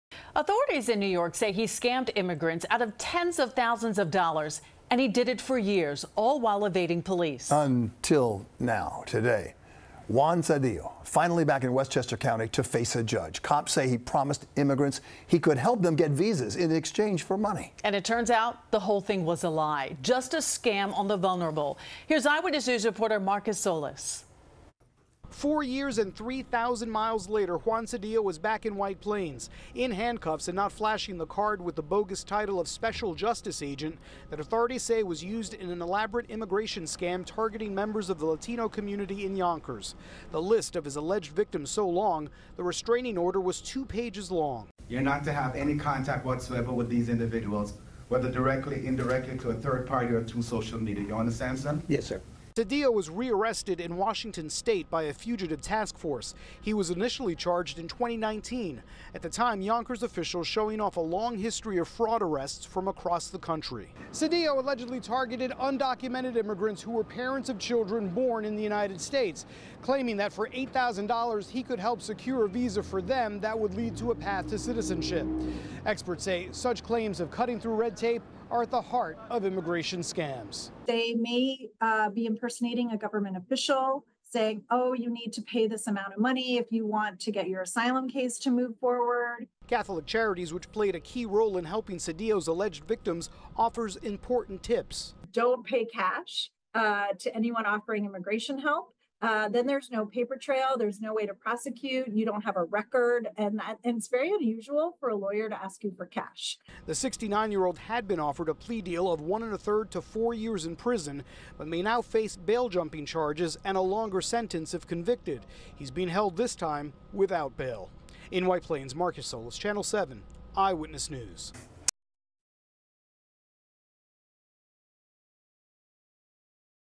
Man accused of scamming immigrants in Yonkers arrested in Washington
Marcus Solis has details on alleged scammer Juan Cedillo, who was extradited to Westchester for his alleged role in an immigration scam.